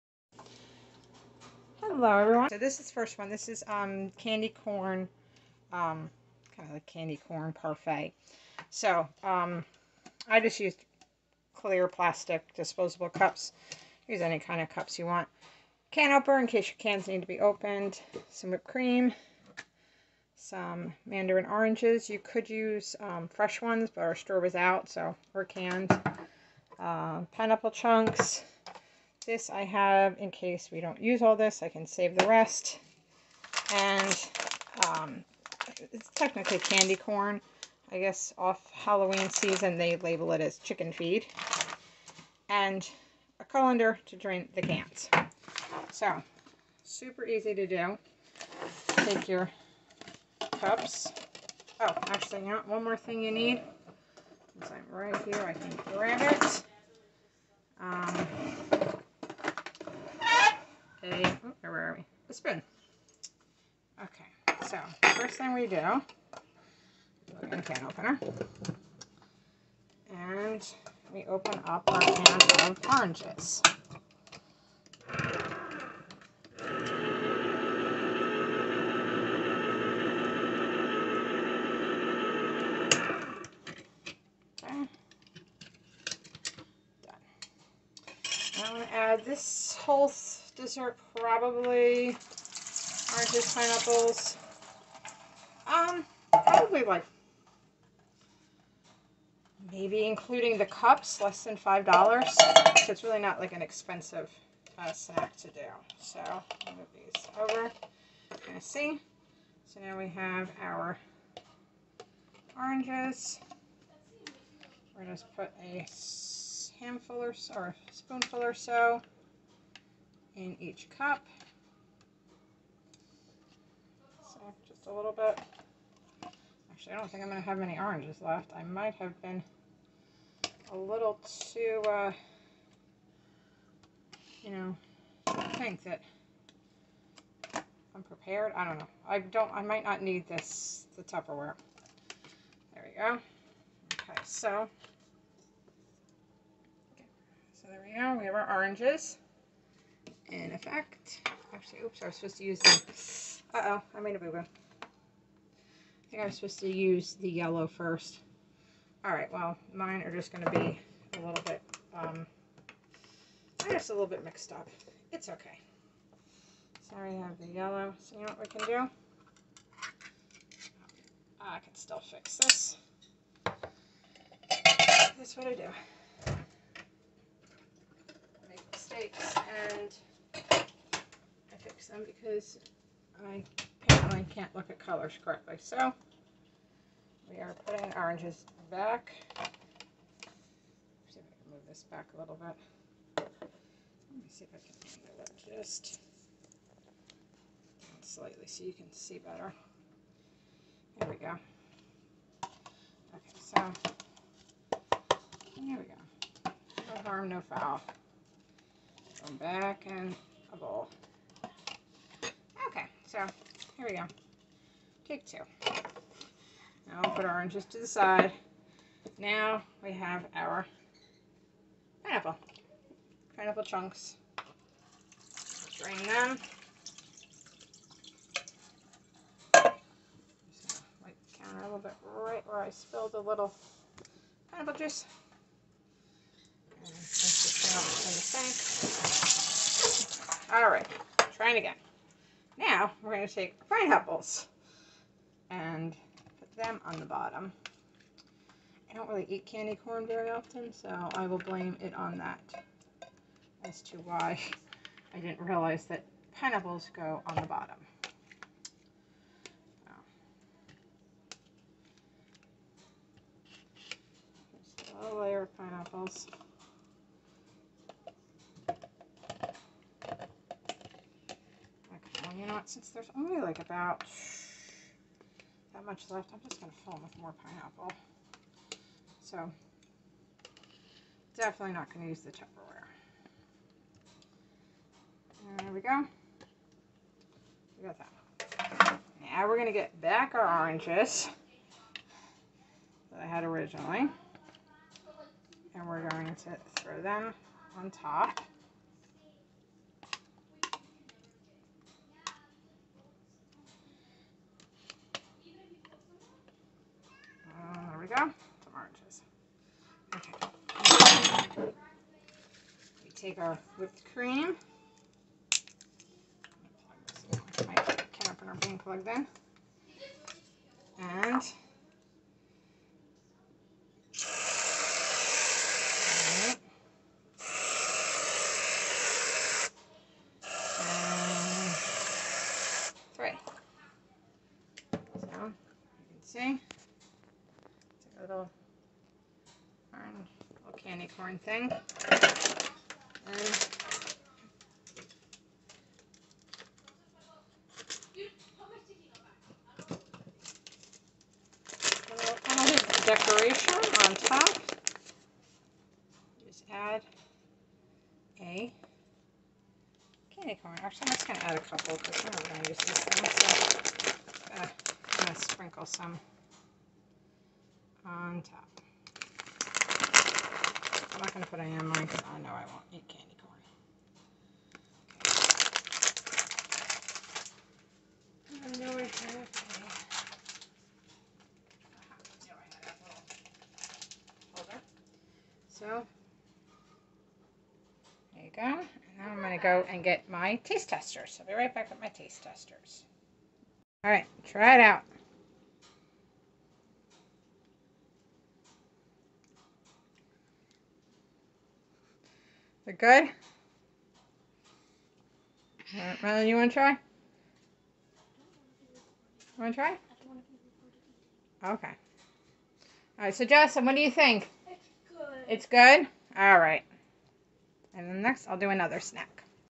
Fun and healthy snack
Candy corn snack. Very easy to make and low cost.